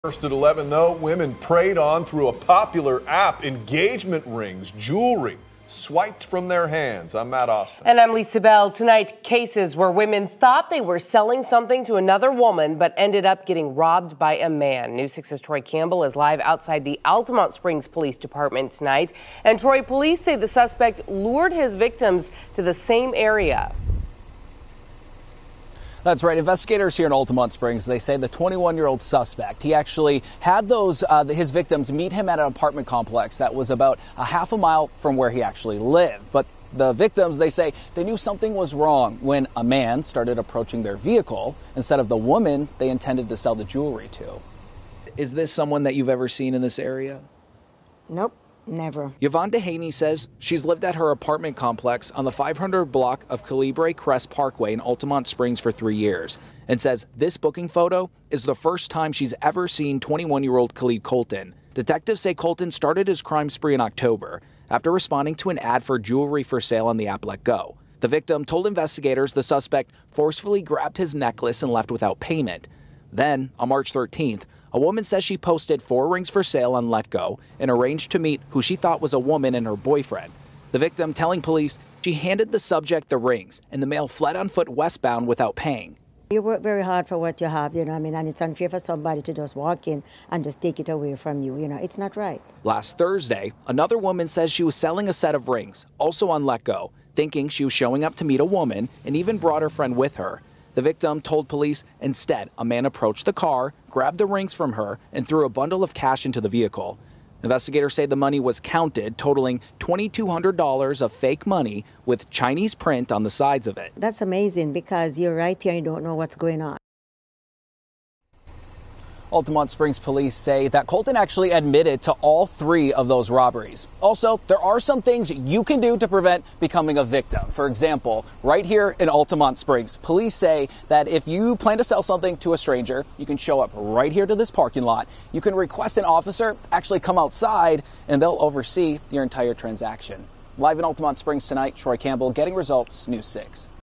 Man arrested for stealing almost $10,000 in jewelry after responding to for sale ad
Police wrote in their report that the suspect lured a couple of his victims to an apartment complex about a half mile from where he lives.